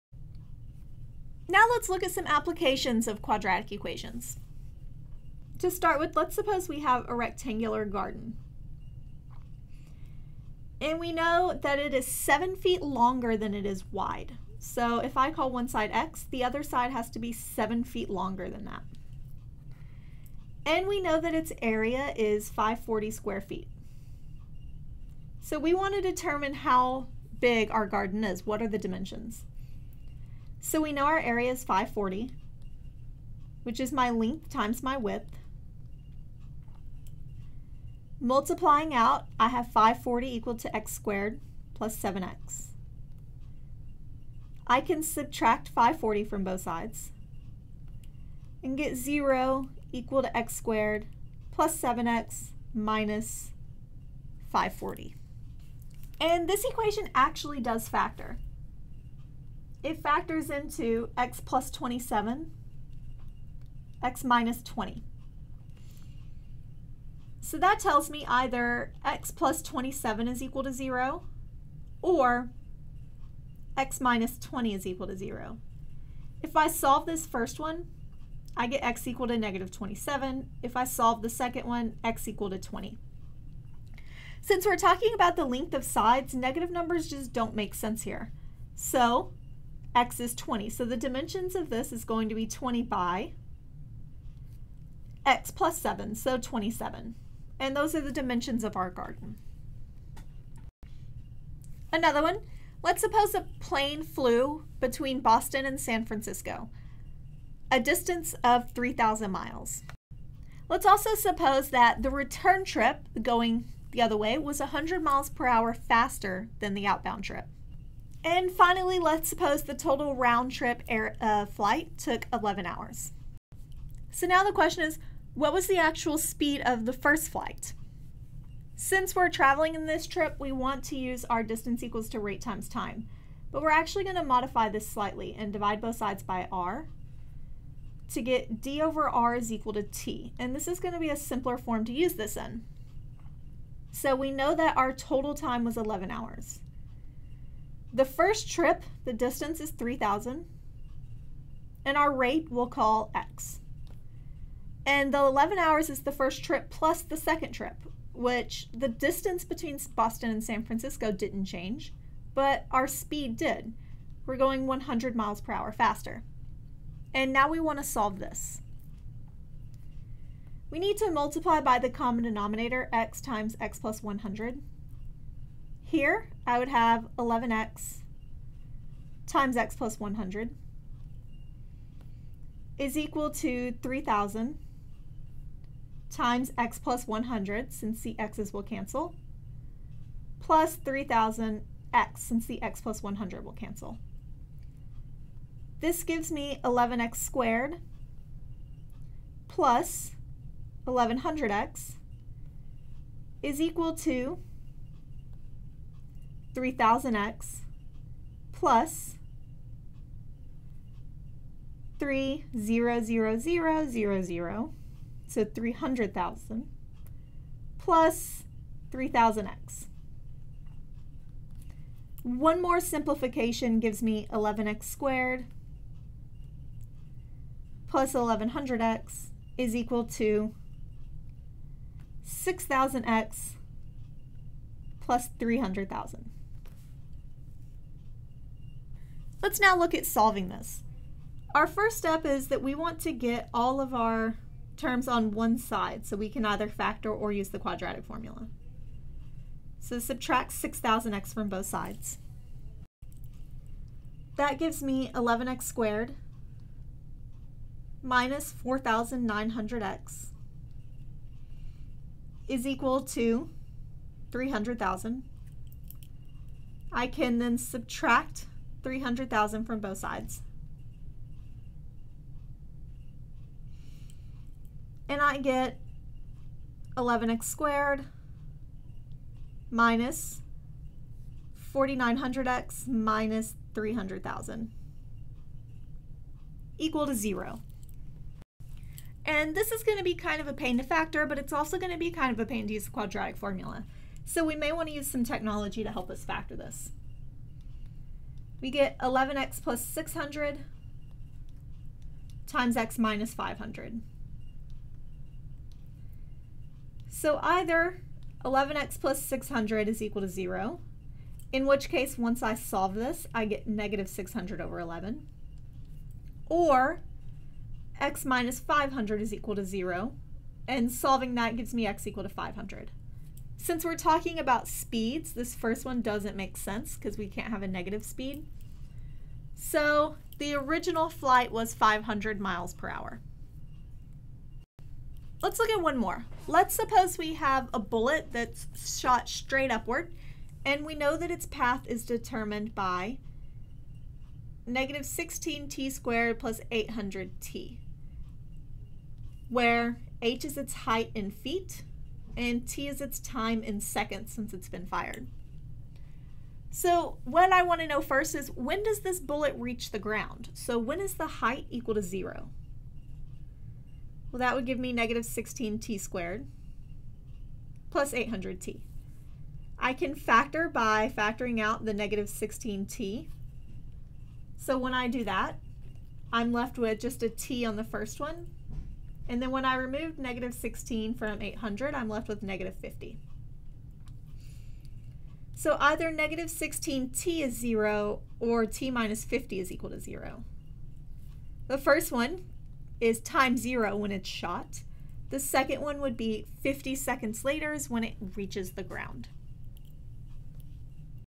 Applications of Quadratic Equations
I show several applications of quadratic equations, and how to solve them.